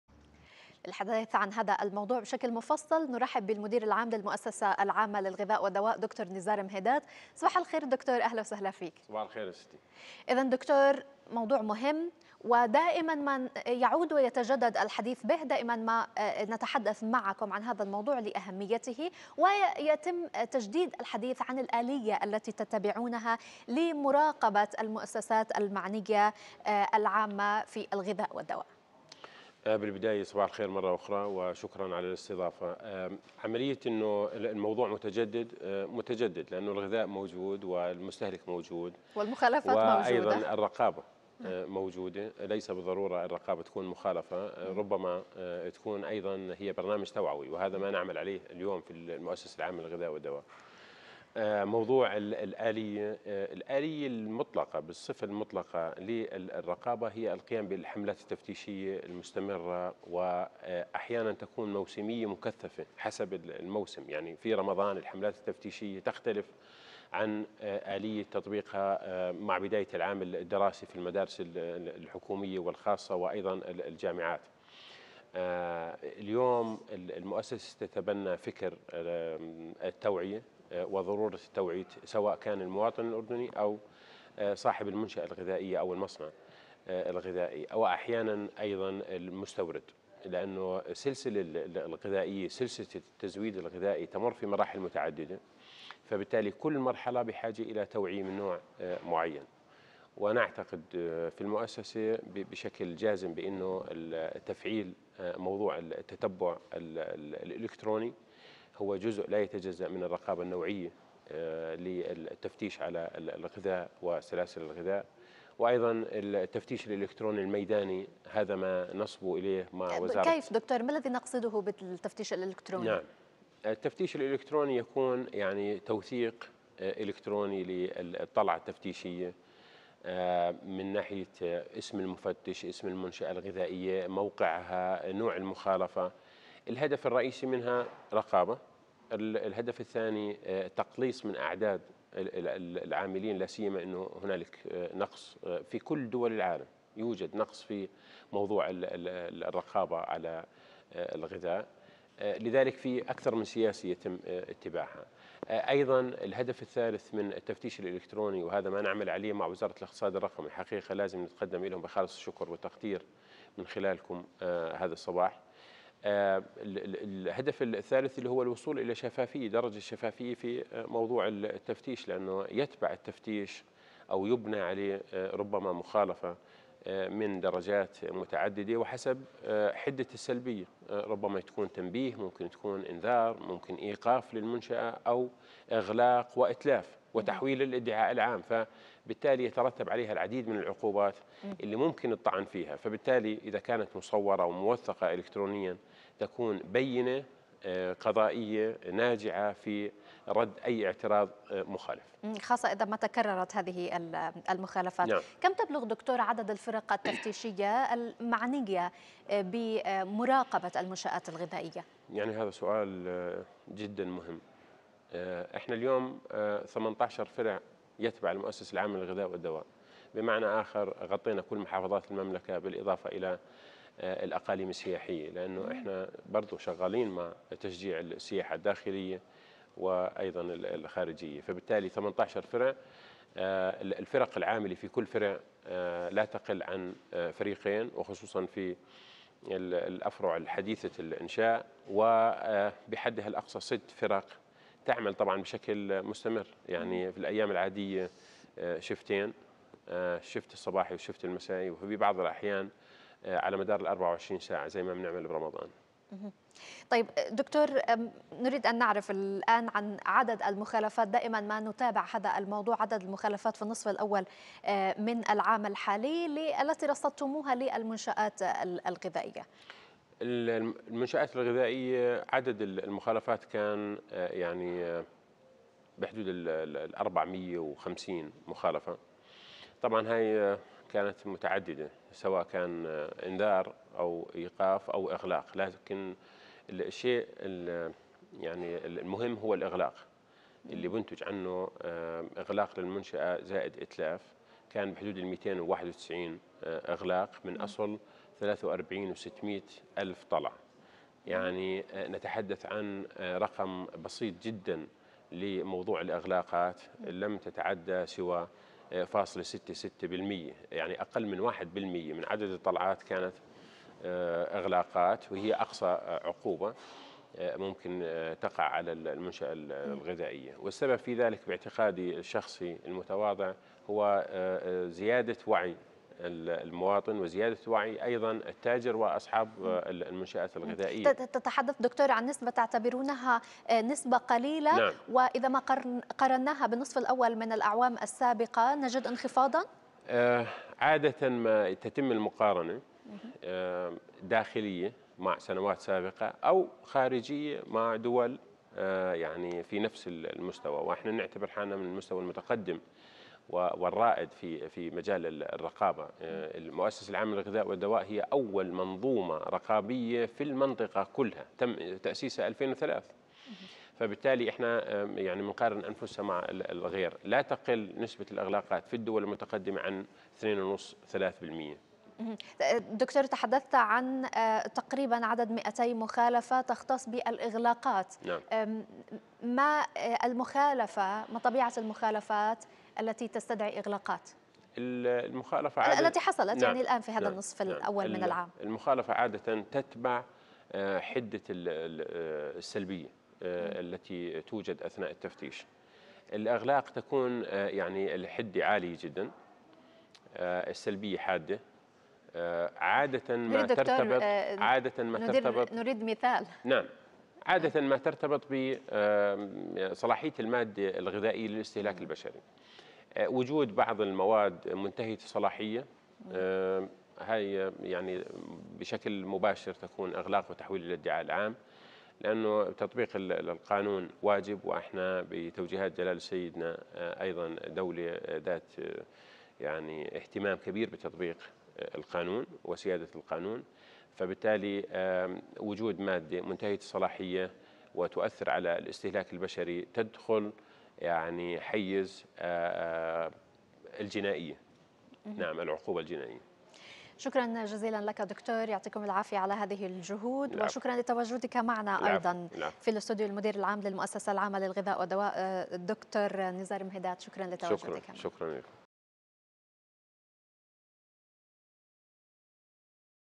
مؤسسة الغذاء والدواء.. آلية الرقابة المتبعة وأبرز المخلفات المتكررة
ما هي الآلية التي تتبعها المؤسسة العامة للغذاء والدواء في مجال الرقابة على الأغذية في الأسواق؟

الضيف:
المدير العام للمؤسسة العامة للغذاء والدواء الدكتور نزار مهيدات

ـــــــــــــــــــــــــــــــــــــــــــــــــــــــــ
AlMamlakaTV
Al Mamlaka
أخبار الأردن
أخبار العالم